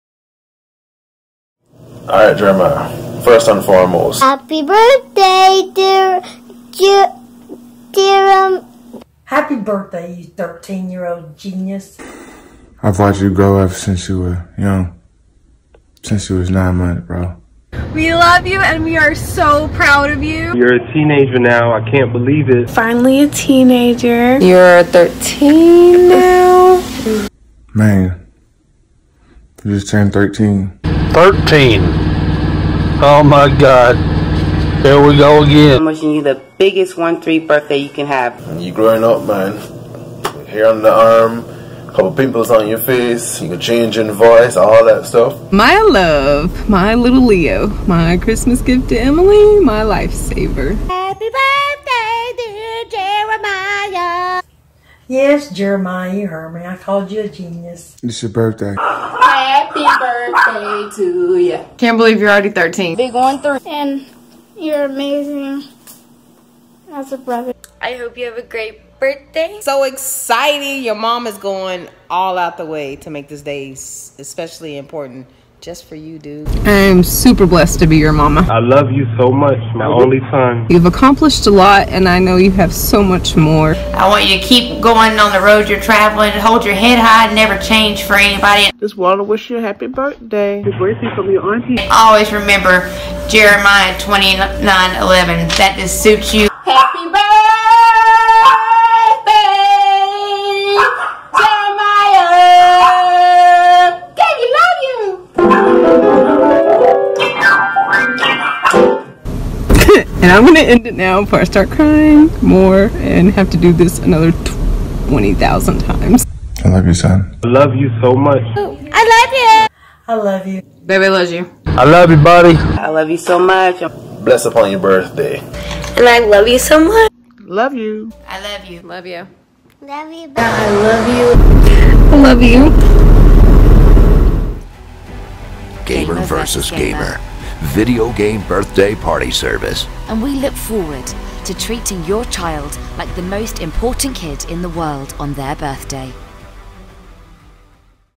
Jeremiah's Atlanta Game Truck Tear-Jerker Video Collage By Gamer vs Gamer Birthday Party Service
Jeremiah's family and friends got together to give him a special Atlanta Game Truck video birthday message.